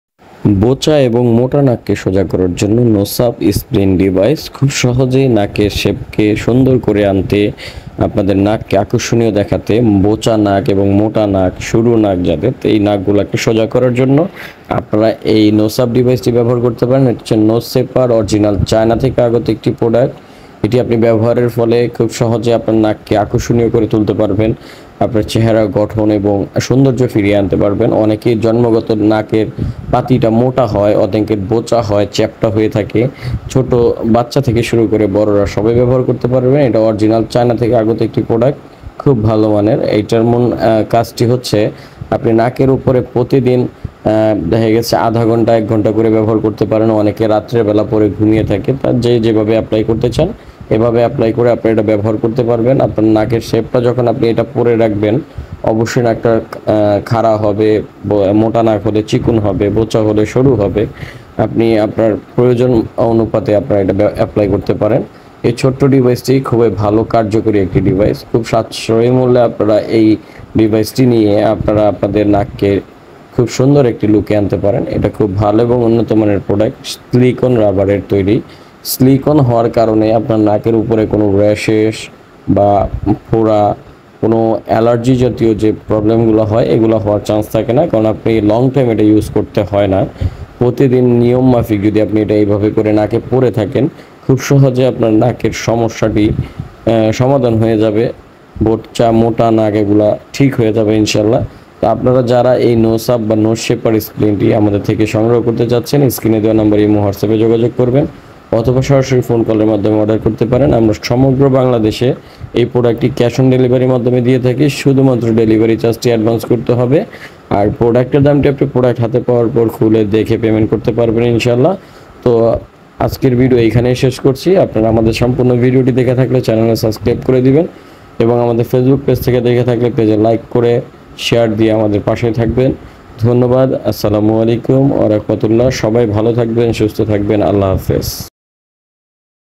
বোচা এবং মোটা নাক সোজা করার ডিভাইস। Nose up splint, Nose shaper nose up control you nose.
Nose shaper /Nose up splint

বোচা এবং মোটা নাক সোজা করার ডিভাইস।
Nose up splint, Nose shaper nose up control you nose.

যাদের মোটা এবং বোচানা চিকন করতে চাচ্ছেন তারা এই ডিভাইসটি ব্যবহার করতে পারেন।

খুবই স্বল্প দামে আপনার এই প্রোডাক্টটি আপনাদের কাছে নিয়ে ব্যবহার করে দ্রুত আপনাদের চেহারা সৌন্দর্য ফিরিয়ে আনতে পারেন।

এটা অর্ডার করার জন্য নিচের দেয়া নাম্বার গুলোতে যোগাযোগ করুন।

Imo-whatsapp ☎️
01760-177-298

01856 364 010